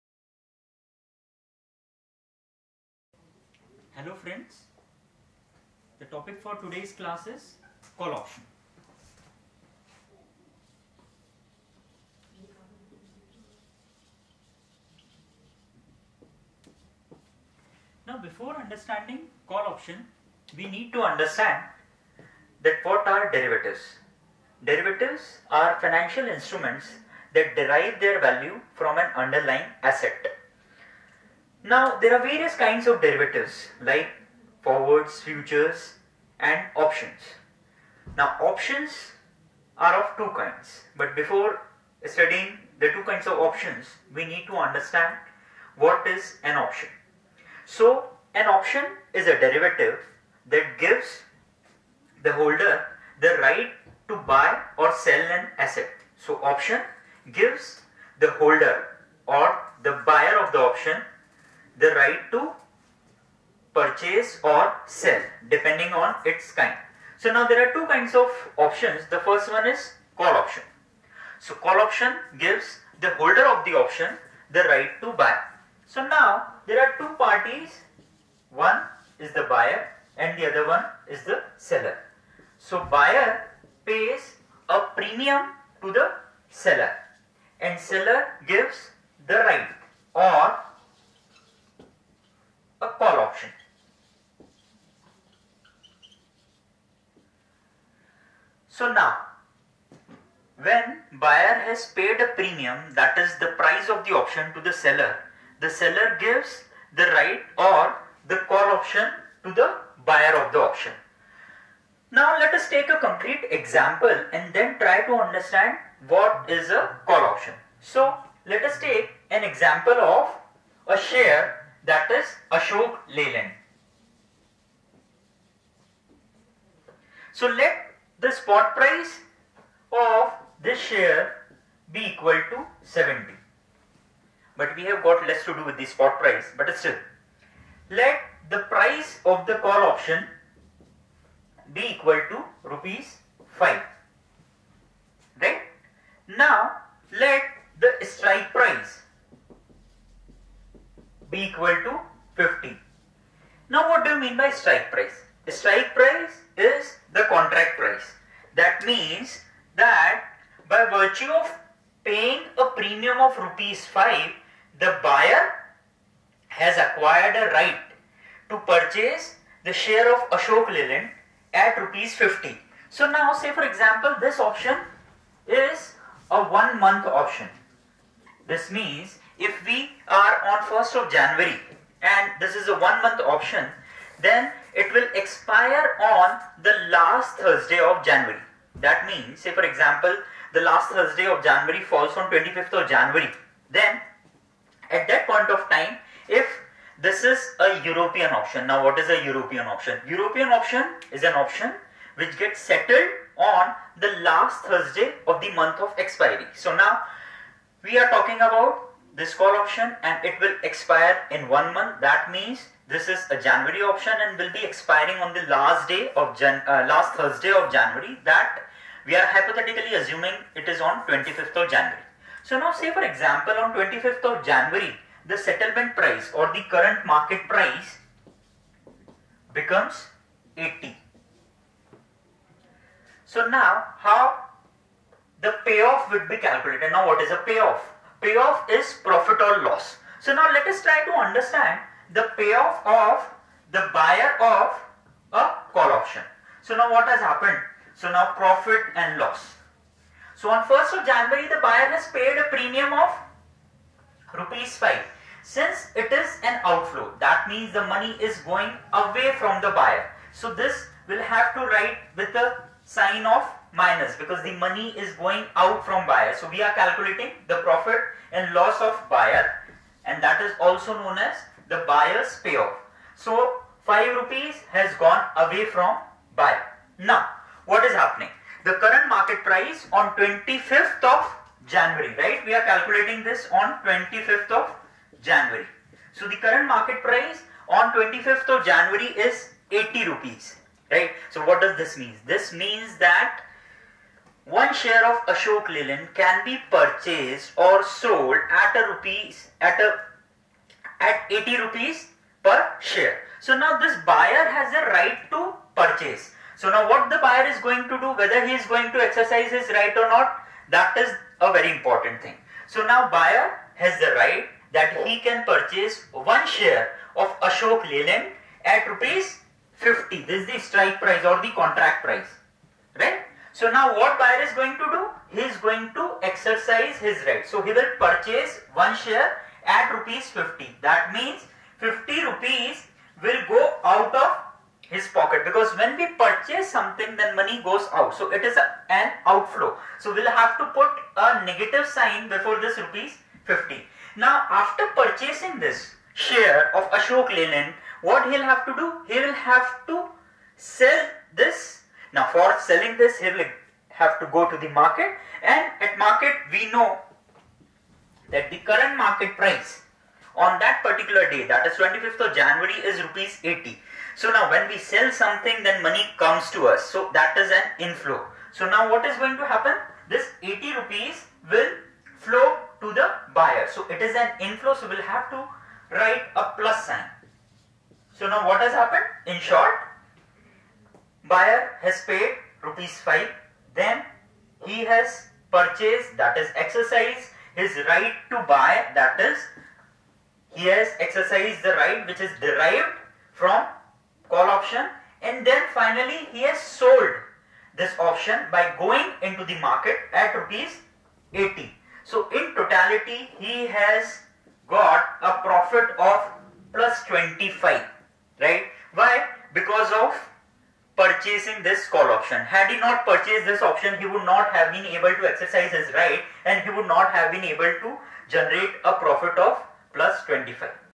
PIMR || Dr. Varun Sarda || Lecture Series || Financial Derivatives || Call Option || Prestige Media
Dr. Varun Sarda talks about Call Option of Financial Derivatives in a very simple, easy and lucid manner in this online video lecture. The topic is explained with real world example.

PRESTIGE MEDIA
POWERED BY DEPT. OF MASS COMMUNICATION

Producer: Prof. Zuber Khan

Session by : Dr. Varun Sarda

Cameraperson: Vedant Singh Bhadoriya & Geetesh Sharma

Editor: Geetesh Sharma